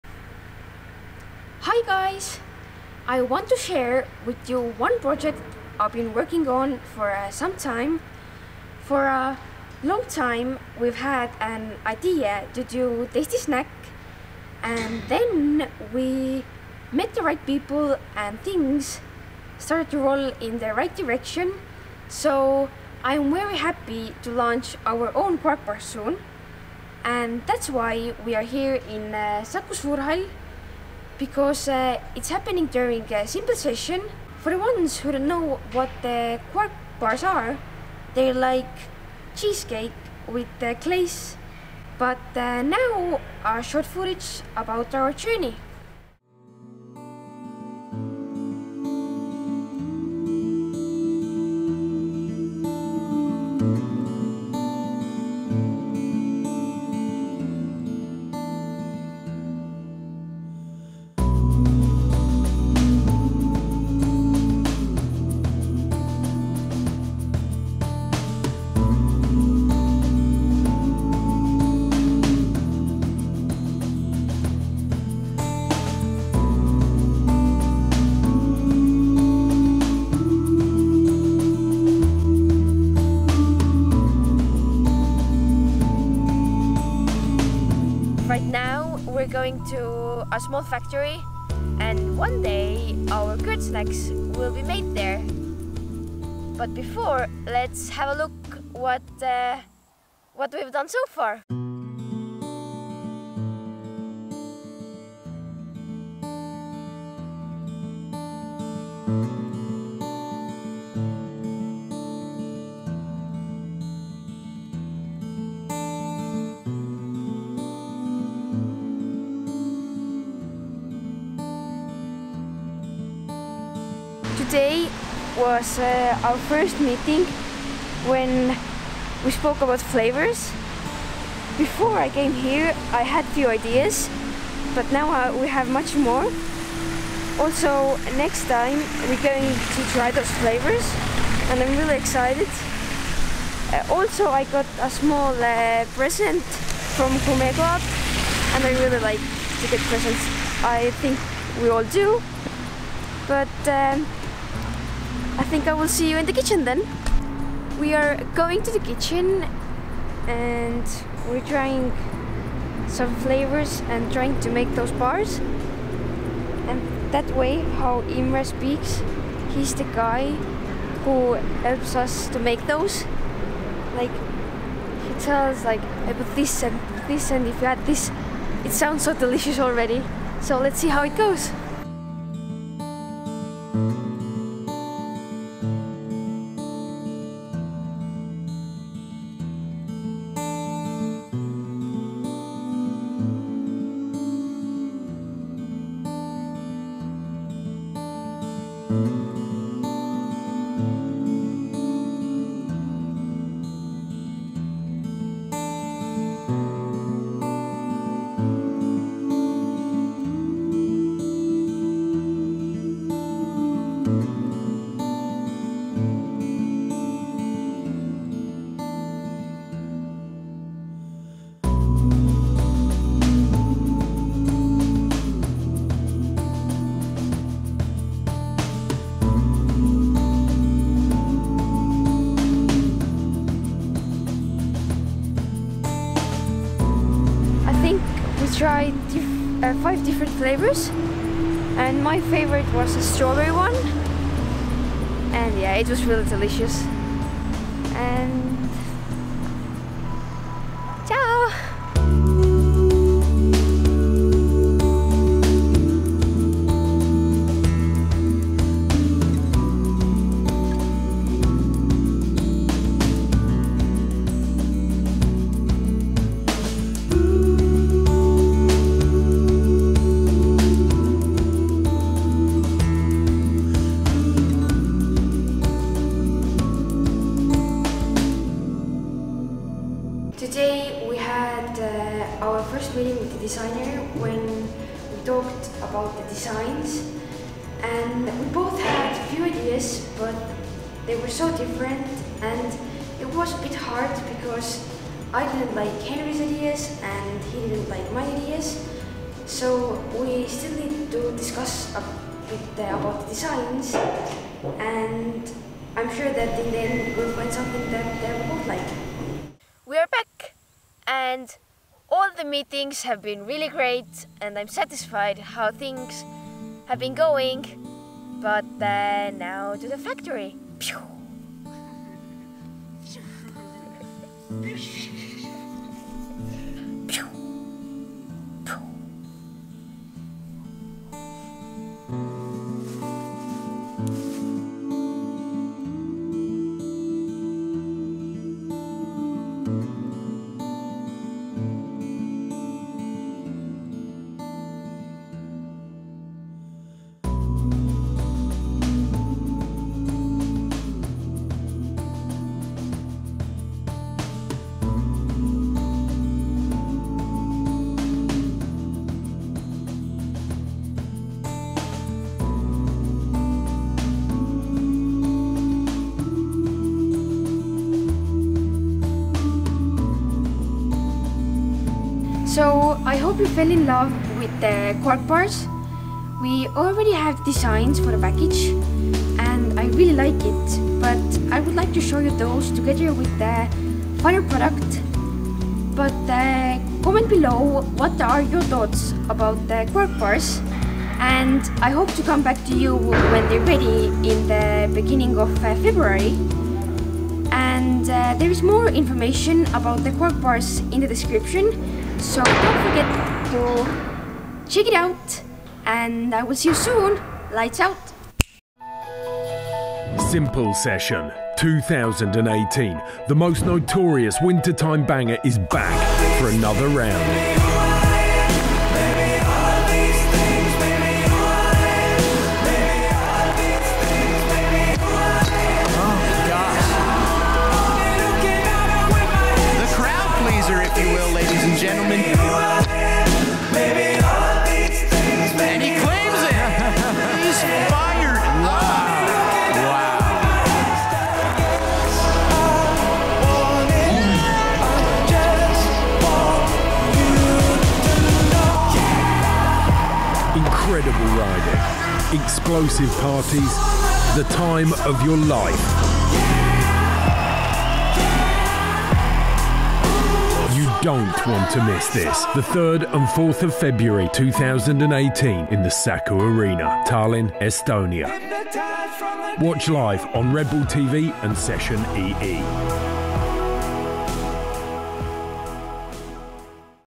HOW WE STARTED OUR OWN LINE OF SNACKS / Vlog1-18
I love to stay active and eating something between the meals keeps me going. I like eating different snacks (in moderate amounts) but dreamed about my own. So, for a long time we have had an idea to do a tasty snack but didn´t get the right feeling or idea to go forward with something certain  until we met the right people in spring 2017 and things started to roll. Together with Estonia's top chef Imre Kose we developed a new high-quality kvarkbar and it was named KellyBar :)  Creating it was fun. We started with baking some home made cakes after our own recipy, Imre studied these and in cooperation with us we developed unique flavors that work well with kvarkbars.

Wait! What is a kvarkbar? Hush ... a kvarkbar is actually a tasty and well-kept secret of Estonia. For 70 years these lovely snacks have been a cult food here – they are loved so much that every Estonian eats 40 kvarkbars a year. Also known as quark, these little treats are made of curd cheese and traditionally glazed in chocolate or caramel. This protein-rich, dairy-based alternative is the cheapest and most convenient way to replace regular snacks.

KellyBars are produced on the ecologically pristine island of Saaremaa. They come with a variety of fillings, such as strawberry-rhubarb, baked apple cinnamon, gooseberry-coconut and cocoa with cookies. All this goodness is glazed in toffee, yogurt, or Belgian chocolate.

Soon you can get them from Estonia but hopefully from other places as well!